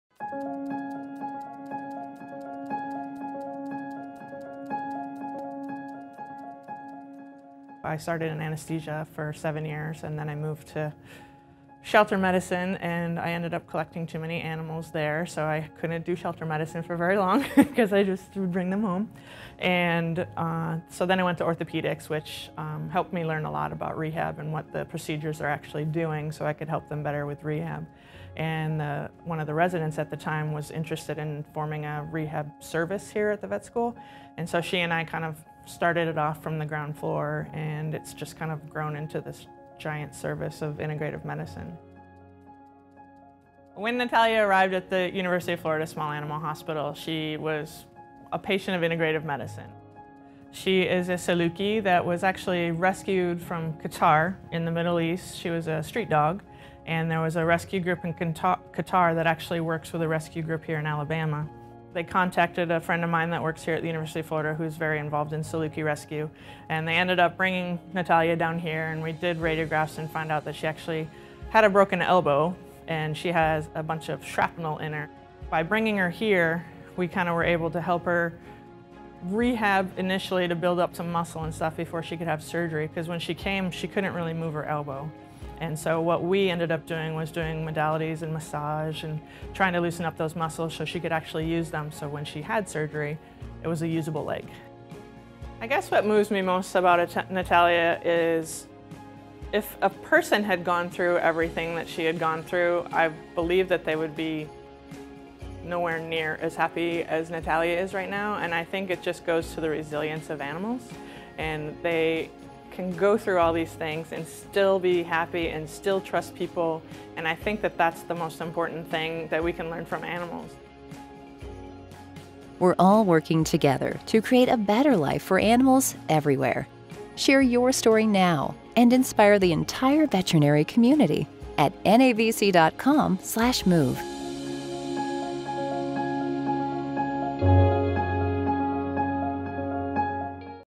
What Moves You | Wendy Davies | NAVC Conference 2016
In this video for the NAVC Conference 2016 "What Moves You?" theme, Wendy Davies shares her story about what moves her as she works with the UF Small Animal Hospital: ""As a professional in the veterinary industry, I get to witness the natural strength and determination of animals. No matter what circumstances they are enduring, they continue to trust us to help them make strides toward a recovery. As I work with my one of my long term patients Natalia, our special bond helps strengthen her muscles as well as her spirit."
- Wendy Davies, CVT, CCRA
UF Small Animal Hospital

See more stories and share yours at move.navc.com.